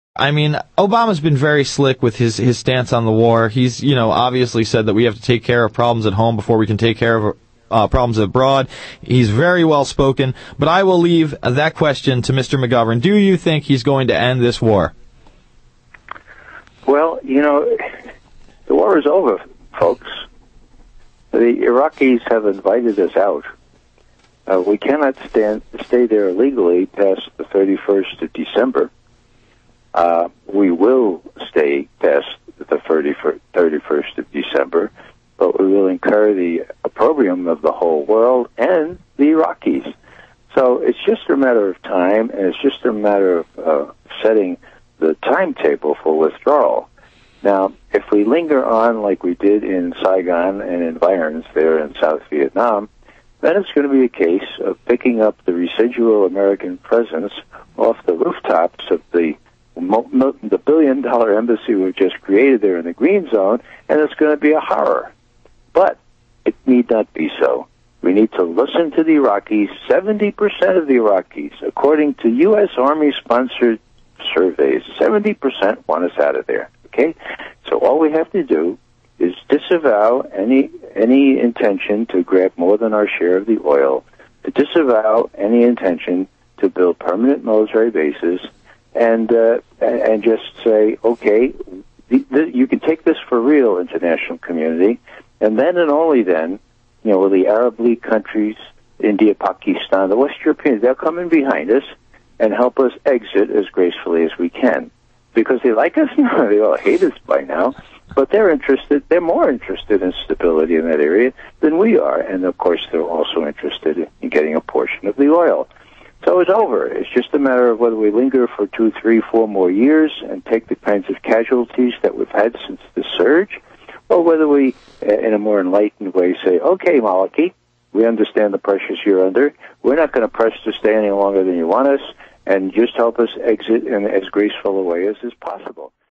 Will Obama End the War?
Obama's ties to many leading military industrial complex investors and profiteers